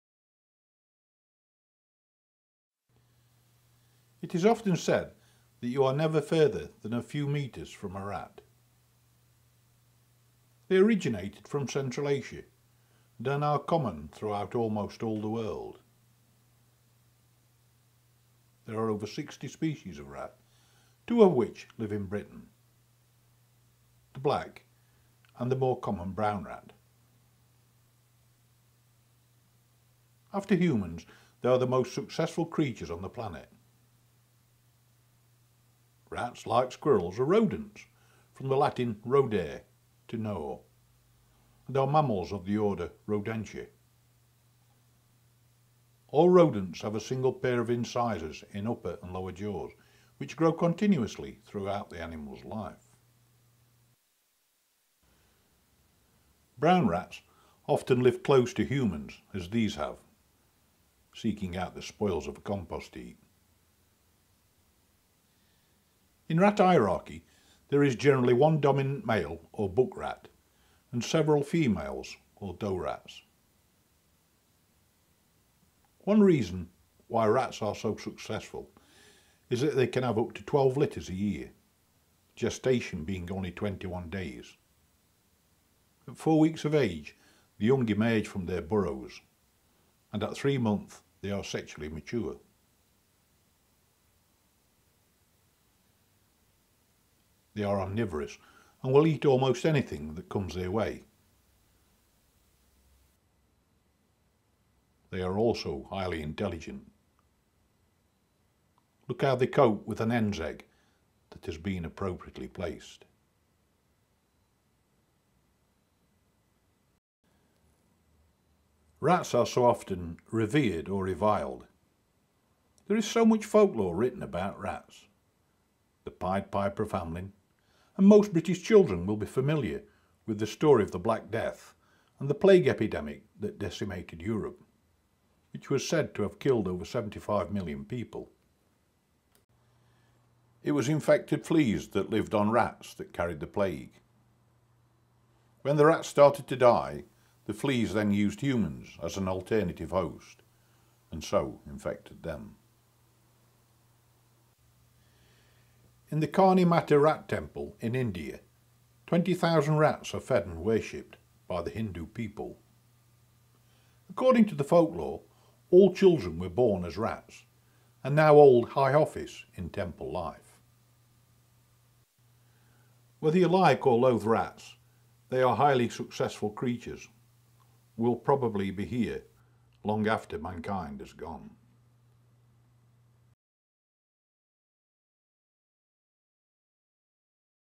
Facts about Rats, carrying eggs and how they live
This video is about Brown Rats, and their young, carrying eggs , climbing trees and a few facts on their behaviour